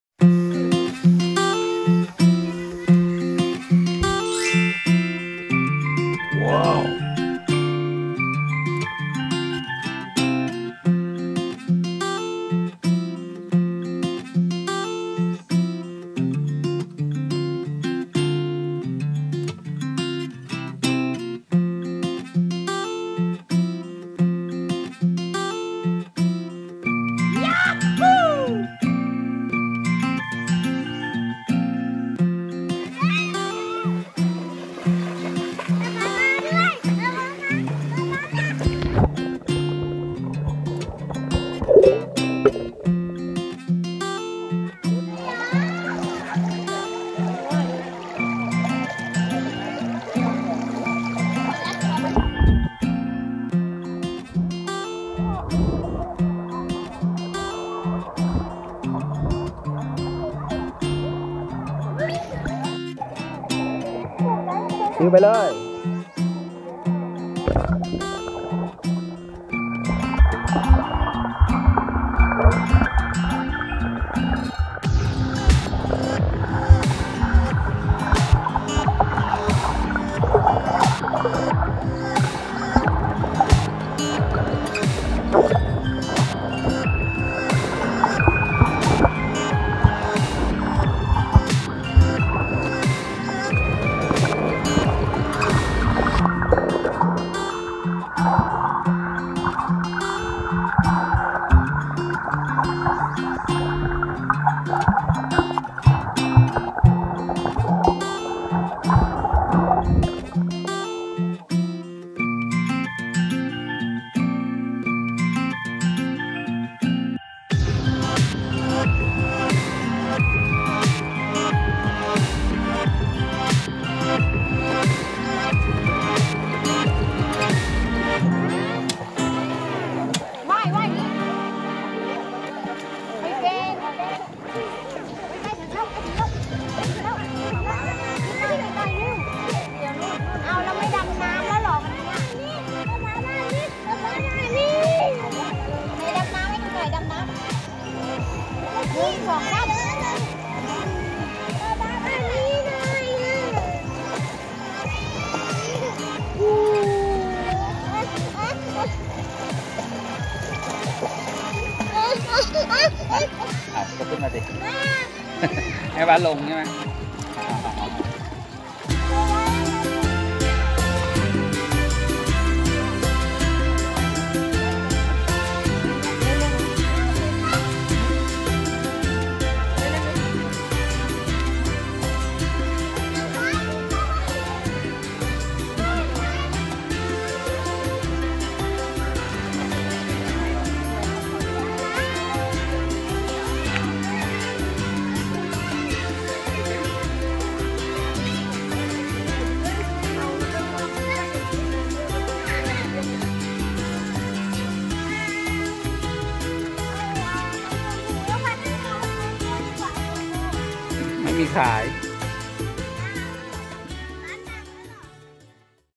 น้องไออุ่น | โดดน้ำ-ดำน้ำ @ป่าต้นน้ำ บ้านน้ำราด สุราษฎร์ธานี
น้องไออุ่นกับพี่ไอคิว เที่ยวป่าต้นน้ำ บ้านน้ำราด จ.สุราษฎร์ธานี ได้โดดน้ำอย่างสนุกสนาน ว่ายน้ำ ดำน้ำ น้ำใสๆเห็นปลาเยอะมาก